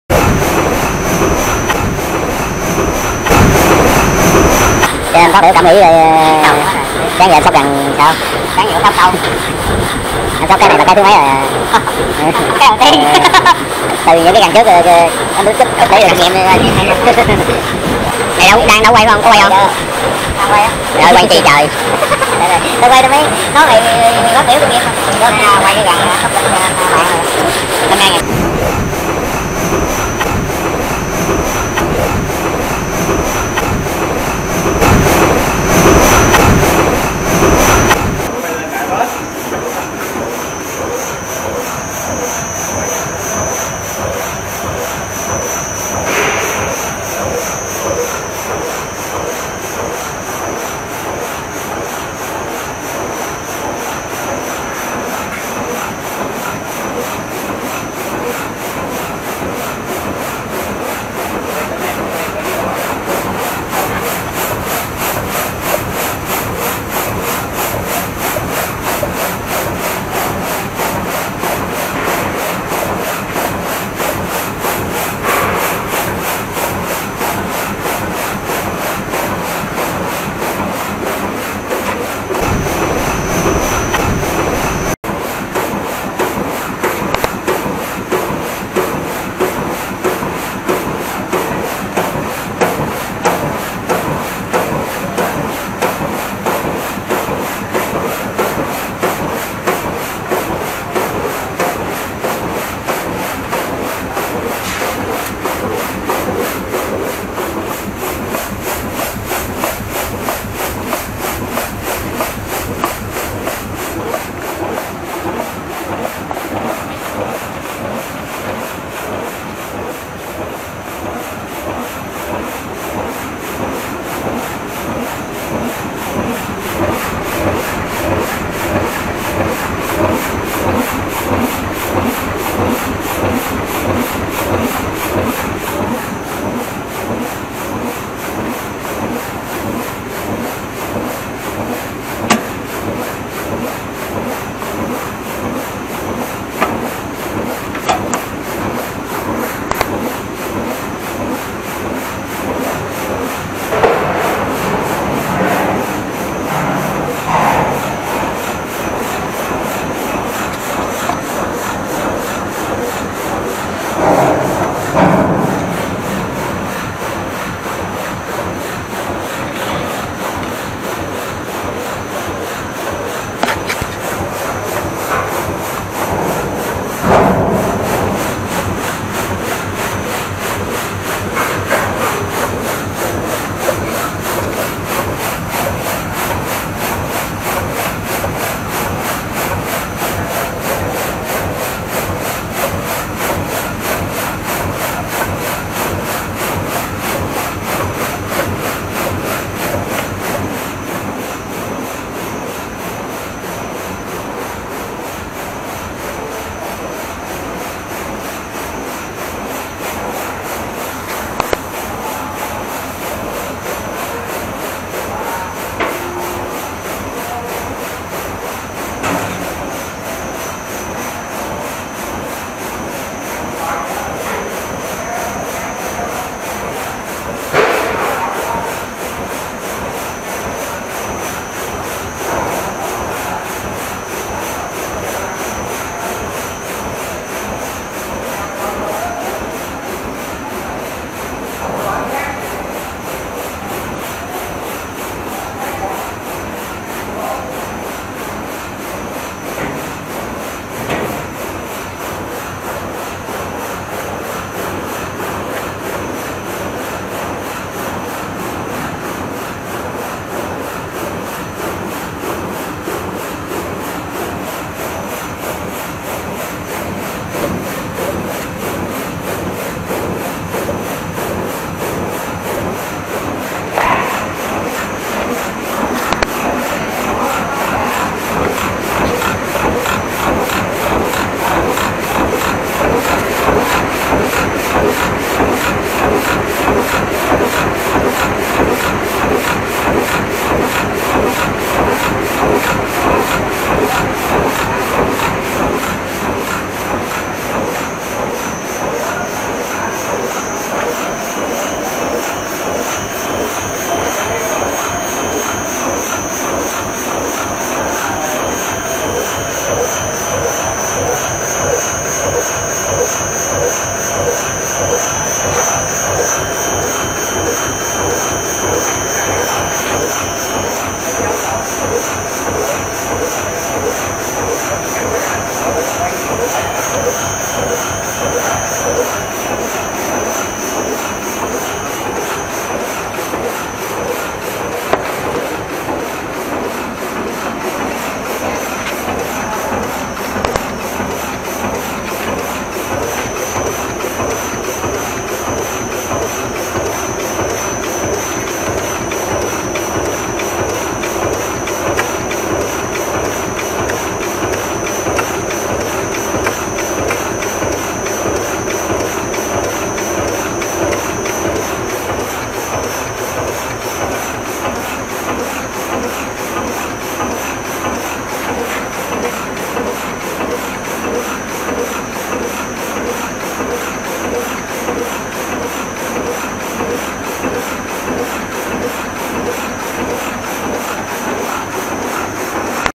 Nghề Cơ Khí - Công Việc Vất Vả Của Thợ Hàn | Xóc Gằn Nhà Máy
nhà máy chuẩn bị đưa vào hoạt động và đây là những công đoạn cuối cùng. mình xin chia sẻ lại cho mọi người cùng xem nhé. chúc mọi người xem video vui vẻ. đừng tiếc một like và một đăng ký ủng hộ mình nhé